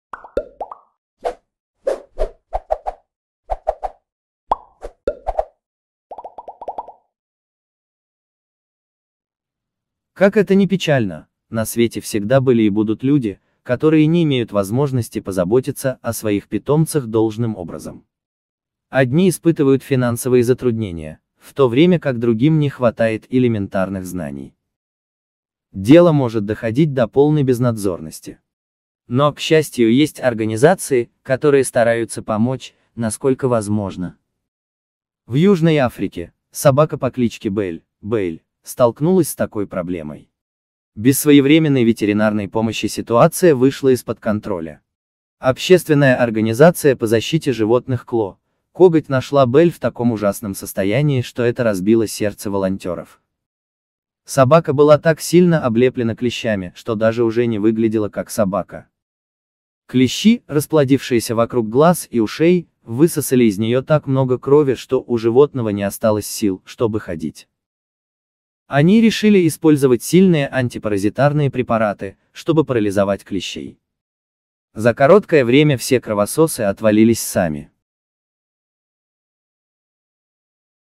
Кровососы атаковали собаку настолько, что медикам пришлось ломать голову, как ей помочь…
Как это ни печально, на свете всегда были и будут люди, которые не имеют возможности позаботиться о своих питомцах должным образом.

Одни испытывают финансовые затруднения, в то время как другим не хватает элементарных знаний. Дело может доходить до полной безнадзорности. Но, к счастью, есть организации, которые стараются помочь, насколько возможно.

В Южной Африке, собака по кличке Белль (Belle) столкнулась с такой проблемой. Без своевременной ветеринарной помощи ситуация вышла из-под контроля. Общественная организация по защите животных «CLAW» (Коготь), нашла Белль в таком ужасном состоянии, что это разбило сердца волонтеров. Собака была так сильно облеплена клещами, что даже уже не выглядела как собака!